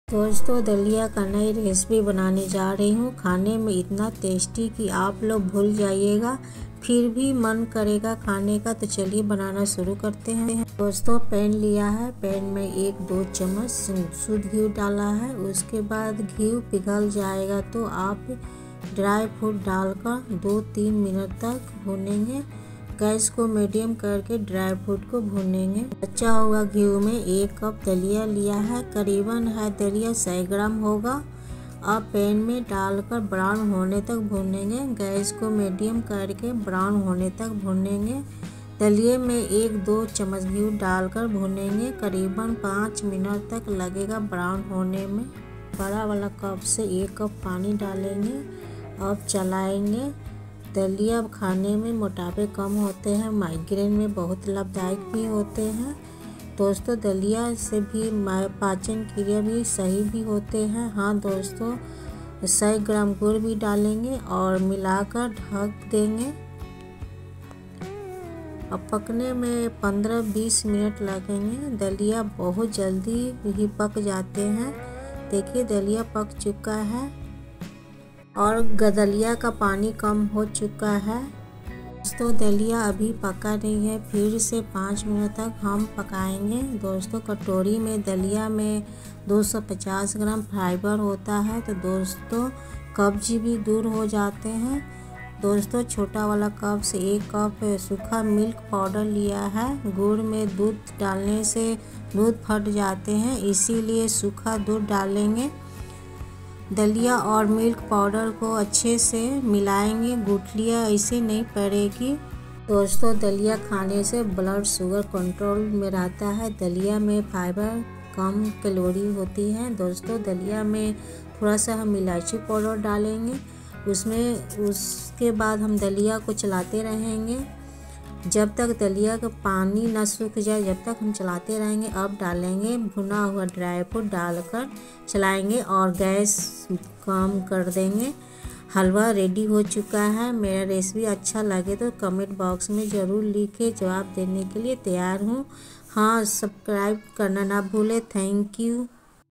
दलिया हलवा बनाने का सही और आसान तरिका |Daliya halwa |daliya halwa Recipe | dalia ka halwa
India#daliyarecipe#halwa#cooking
Must Read to Ten YouTube video viral जिनसे आपकी वीडियो वायरल होगी |

1 bowl daliya (150 gram)
1 bowl jaggery ( 150 gram gud)
1/4 tsp cardamom powder
2 tsp ghee
1 liter water